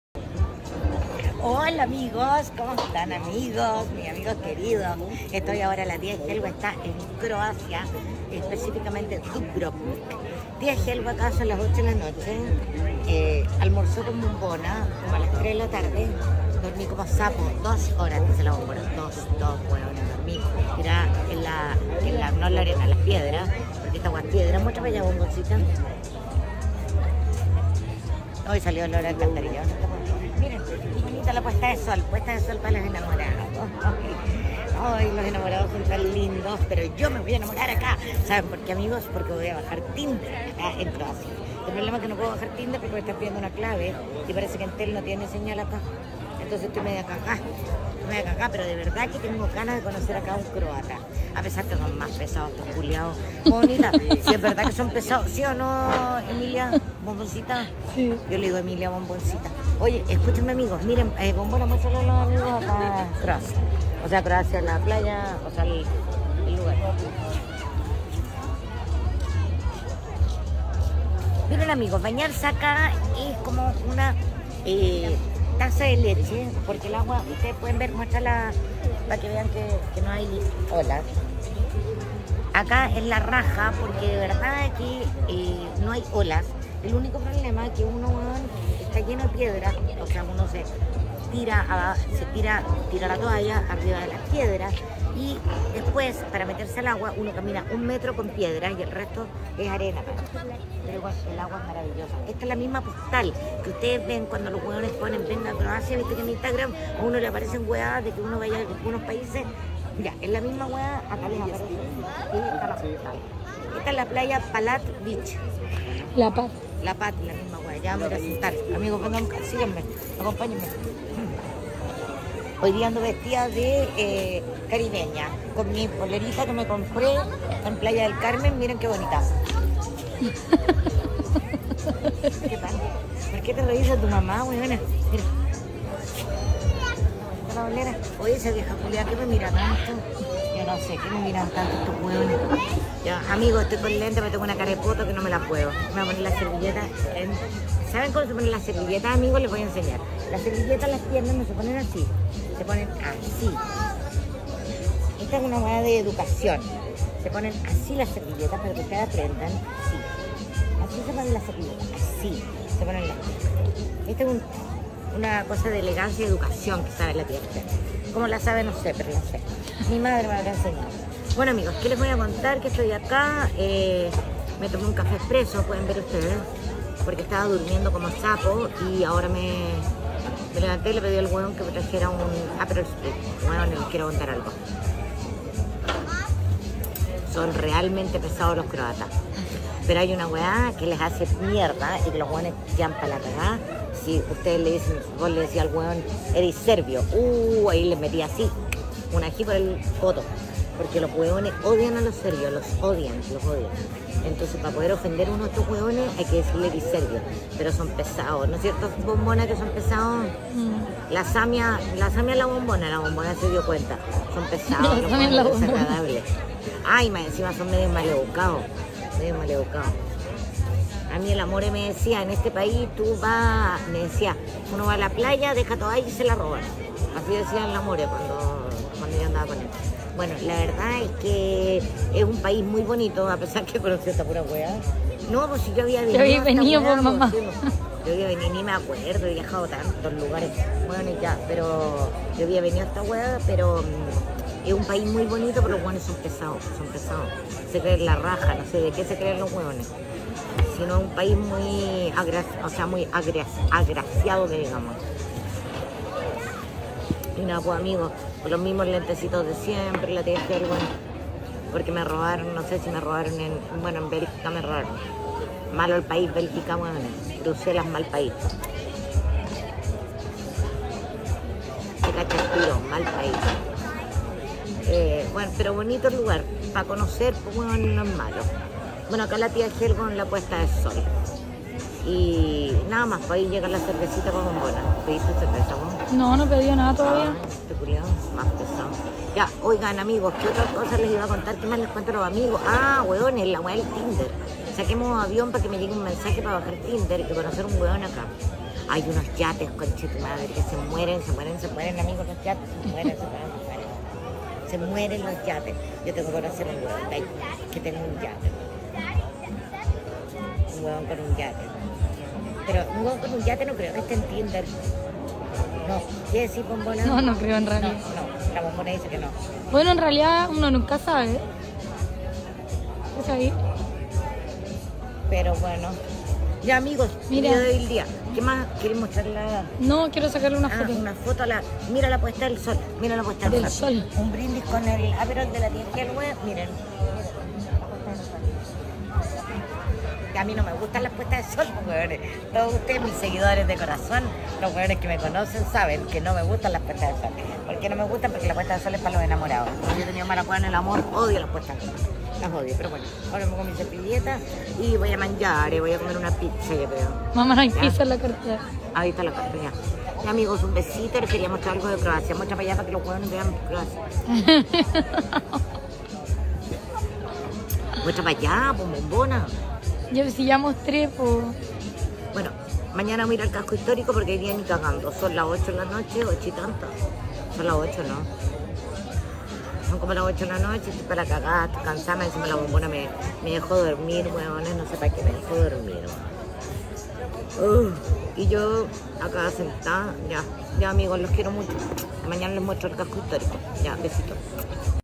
HELHUE SUKNI DESCARGÓ TINDER PARA ENCONTRAR PAREJA EN SUS PARADISIACAS VACACIONES EN CROACIA
Helhue Sukni y su hija, Samia, a quien nombra habitualmente como la “Bombona”, se fueron de Francia y por estos días disfrutan de sus vacaciones en Croacia, donde la abogada descargará Tinder. Así lo aseguró en sus redes sociales en las últimas horas, destacando que llegaron hasta Dubrovnik, donde almorzaron y durmió una siesta de dos horas, que se le fue de las manos.